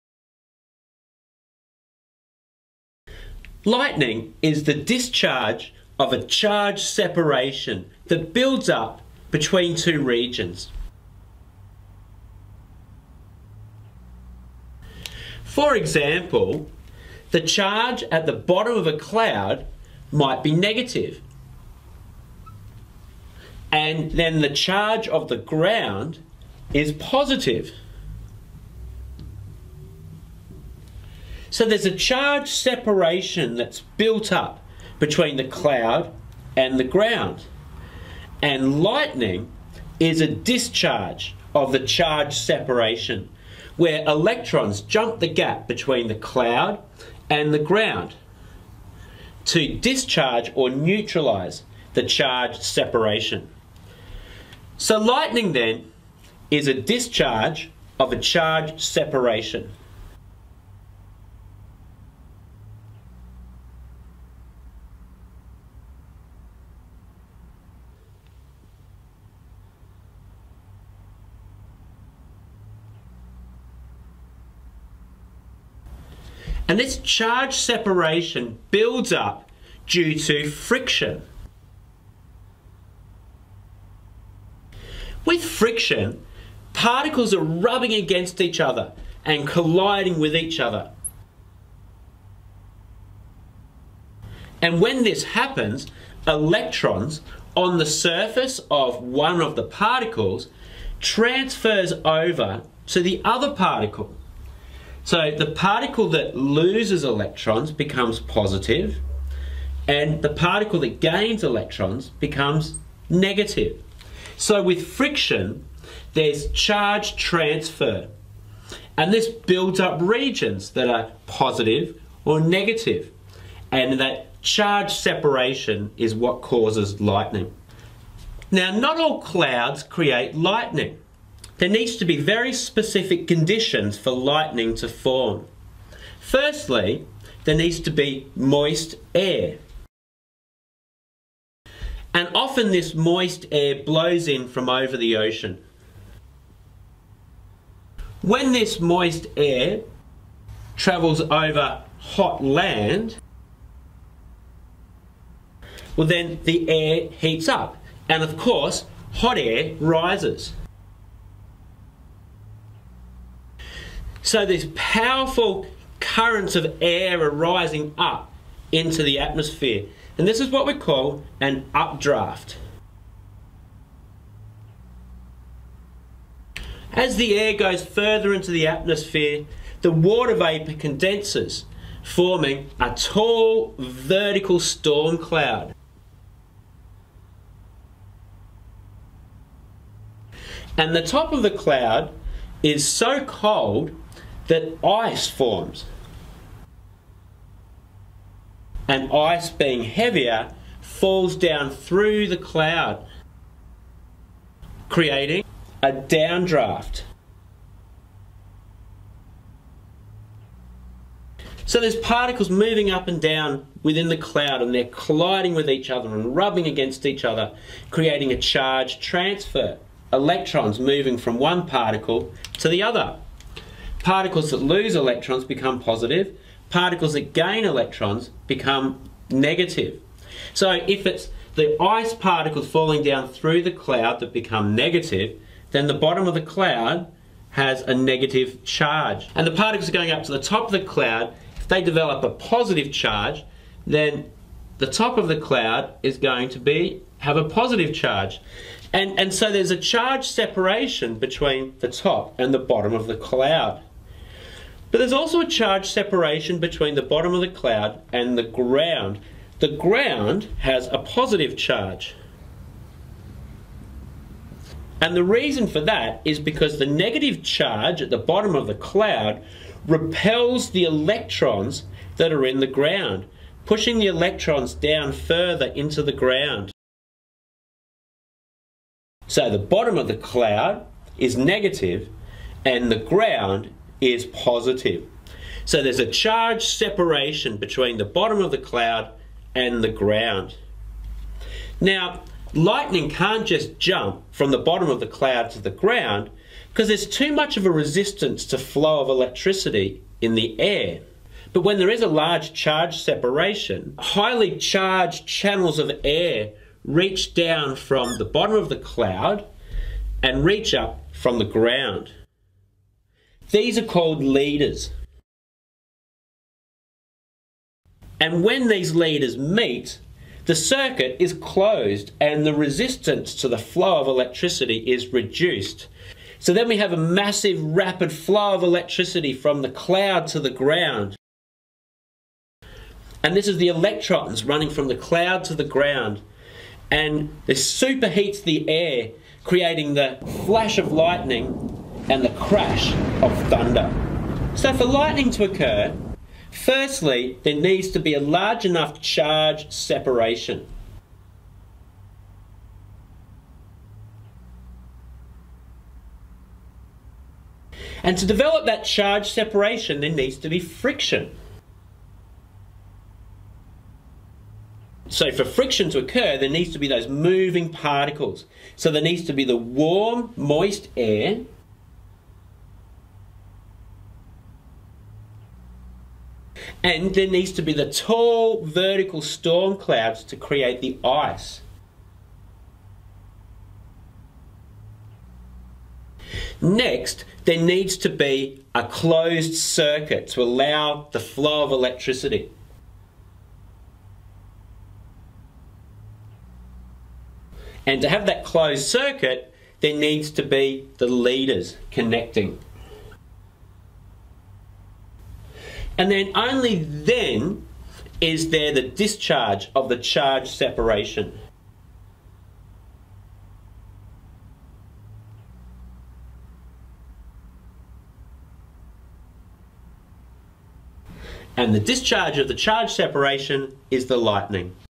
Lightning formation
This video describes the process of lightning formation